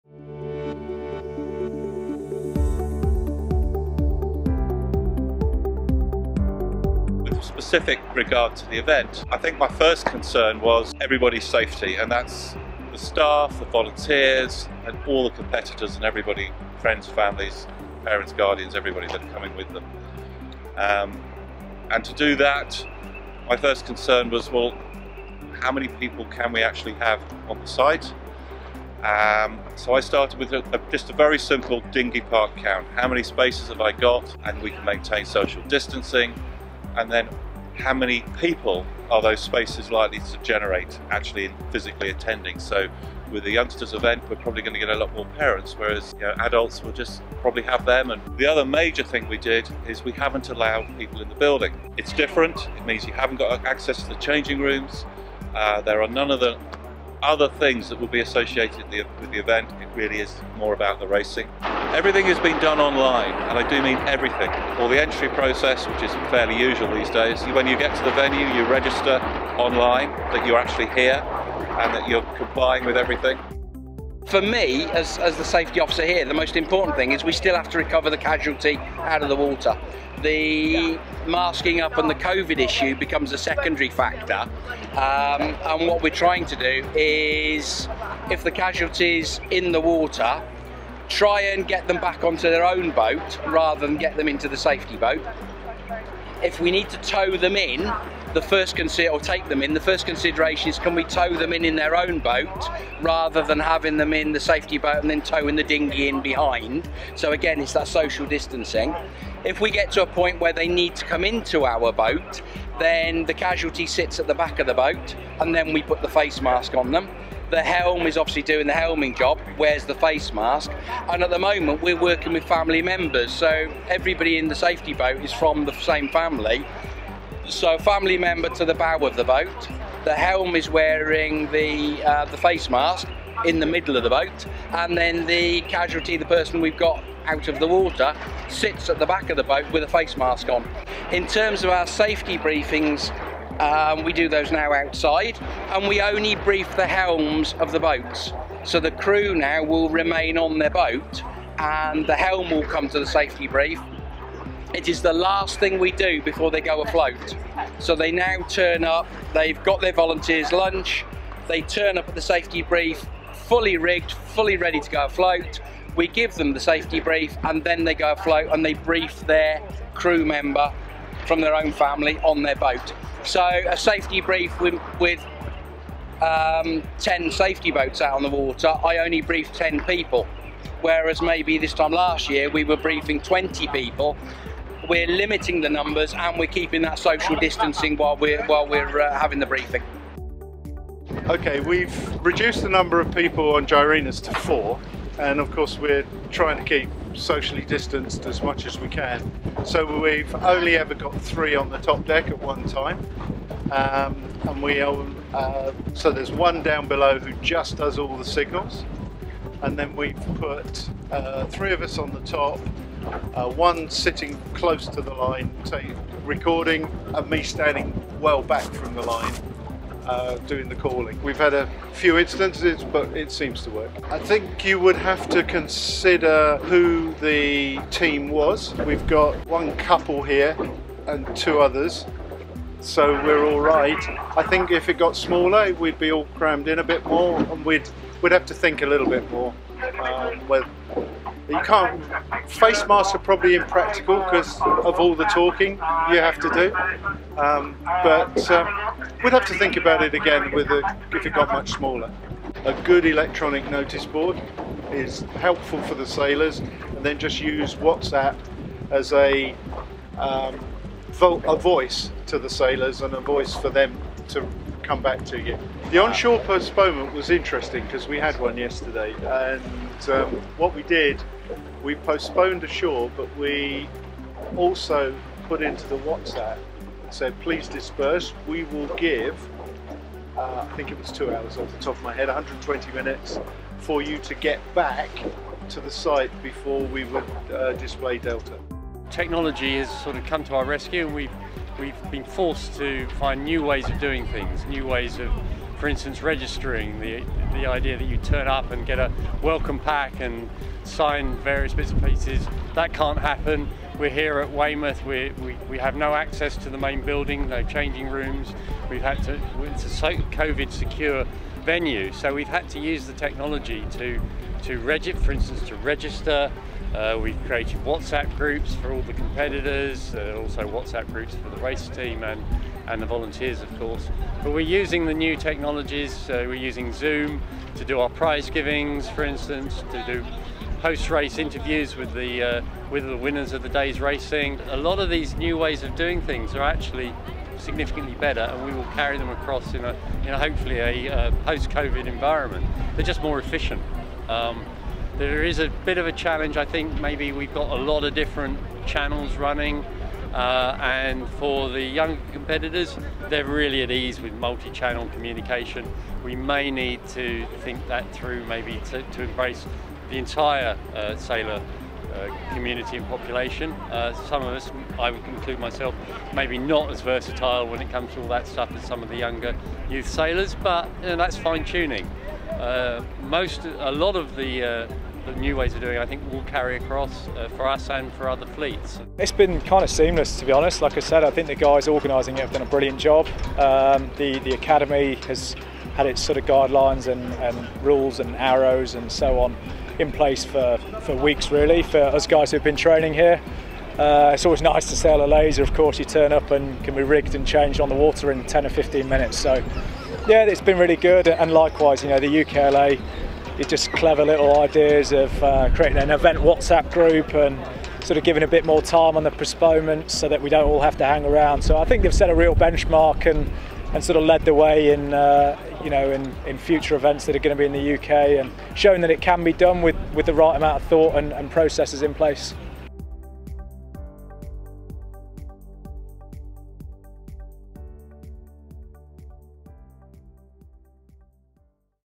EVENT PROFILE - ILCA ( LASER ) NATIONALS 2020 - Running an Event Post Coronavirus
0:00 - Introduction
0:06 - What did the Venue do?
1:01 - How does Entry and Registration work?
1:13 - How does Safety Cover differ?
2:20 - How do Safety Briefings differ?
3:10 - How did the Committee Boat differ?
3:44 - What if out Committee Boat is smaller?
4:11 - How did you keep the sailors informed?
4:49 - How has technology helped you?
5:34 - What does the future hold for sailing regattas?
6:19 - What do the sailors think?

The UKLA ILCA ( Laser ) National Championships took place in Weymouth between 24th - 29th August, but what does it take to run a regatta in a post-covid world? We spoke to some of the key organisers of the events to find out how they pulled it off and what changes they had to make.